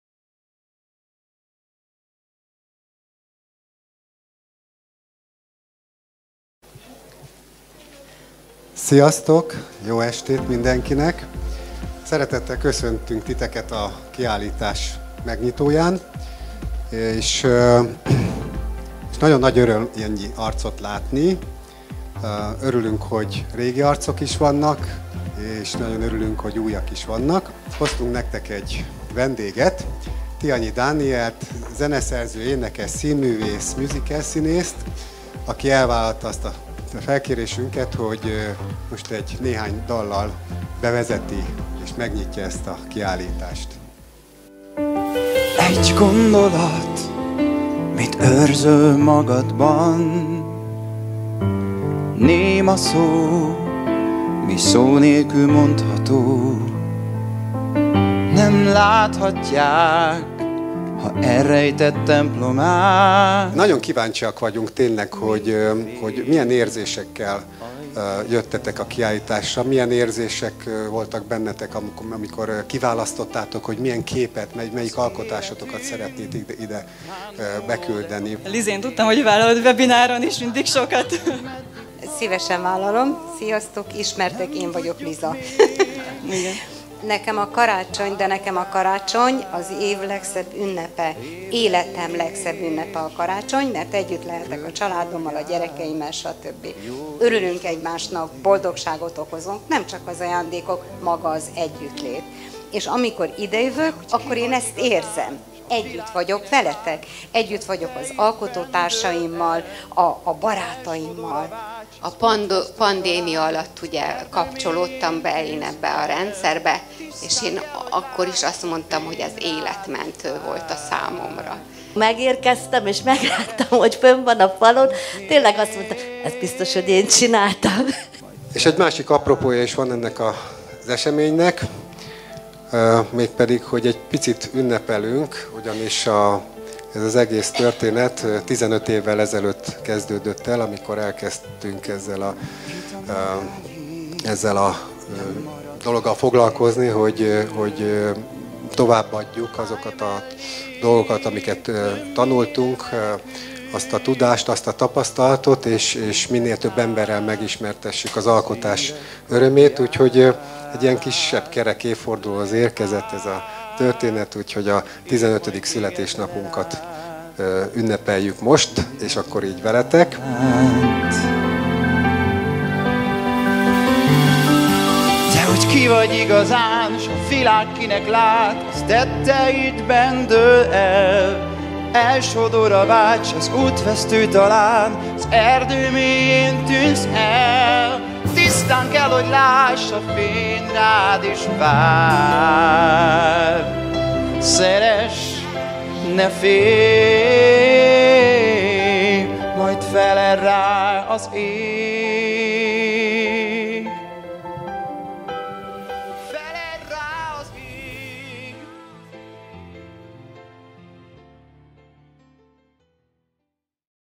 ValDor Art "Alkoss szabadon" Mentorprogram évzáró kiállítás összefoglaló videó
Ebben a rövid videóban visszanézheted a Valdor Art "Alkoss szabadon" Mentorprogram évzáró kiállításának legjobb pillanatait!